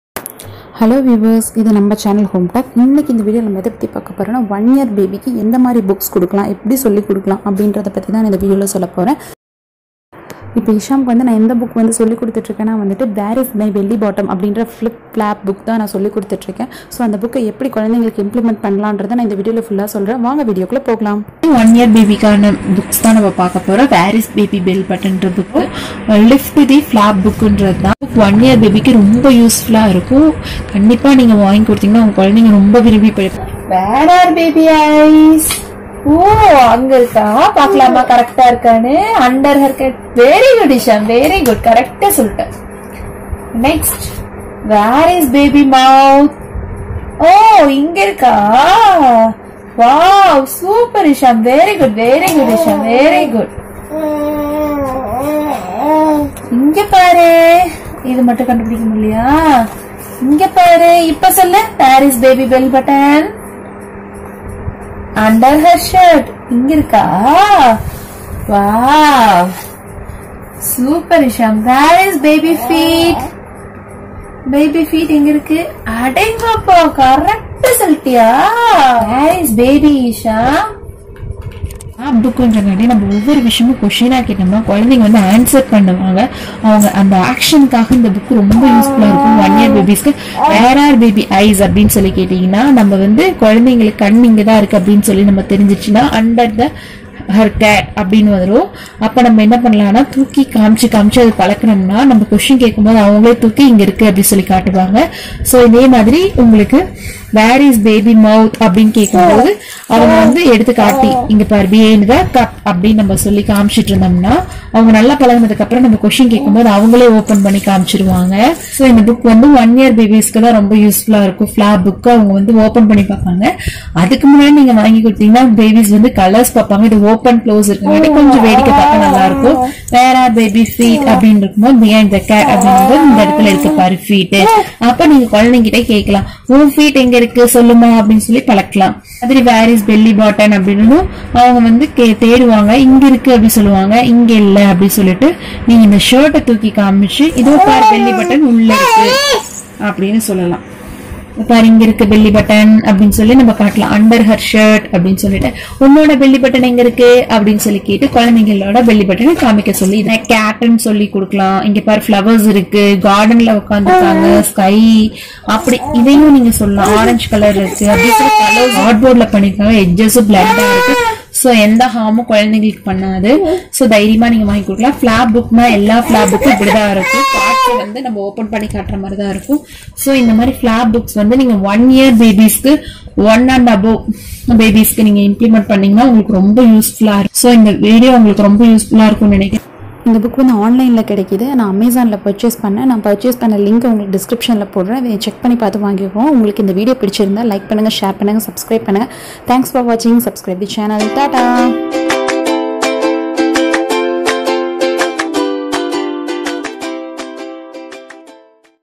Where is baby's Belly Button? Book for Toddlers 📓📒
In this video describing how to use belly button book amazing for kids. It is very useful to Kids  and they have fun with this book while reading.

About this book:
Where Is Baby’s Belly Button? offers a fun and innovative way to learn about the different body parts. The little ones will have the most amazing time finding out answers to questions such as “Where are the baby’s eyes?” and “Where are the baby’s hands?” Kids will have to lift the flaps to get answers to the questions. The book features round-faced, cute looking babies of different nationalities. Beautifully illustrated and very appealing, Where is the Baby’s Belly Button? is perfect for little kids.

This book is available in Amazon and Flipkart.